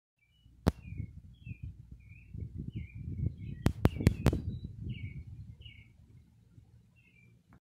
Redneck with a Palm Tree
Here's a Palm Tree I got online. It's sitting next to the rose bush I cultivated over the winter. Feel free to loop the video.